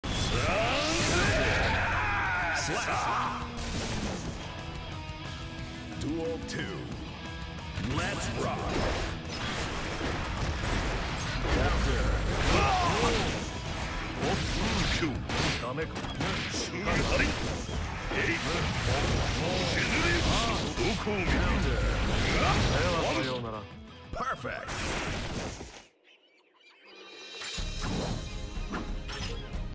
This Zato was not Ready GG
I think he won the set afterwards! but this Perfect was clean, GGs tho!